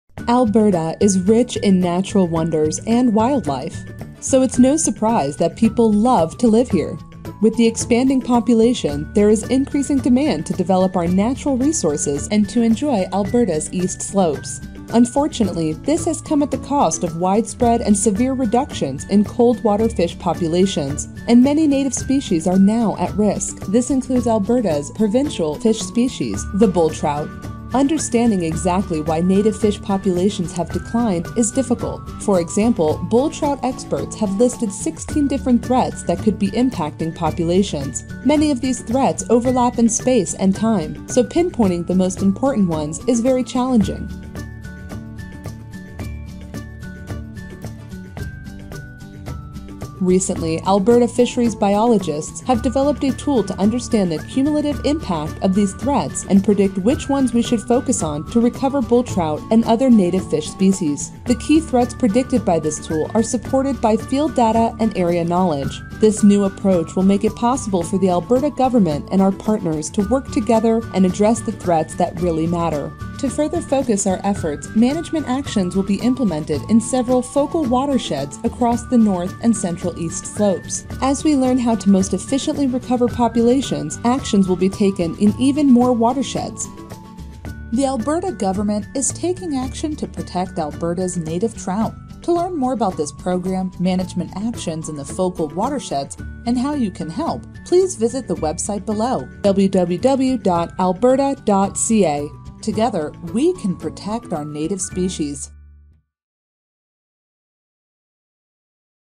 Alberta’s Native Trout Recovery Program
Alberta's Native Trout Recovery Program is a comprehensive, long-term fish conservation initiative aimed at monitoring and recovering populations of native trout and whitefish in the watersheds of Alberta's eastern slopes.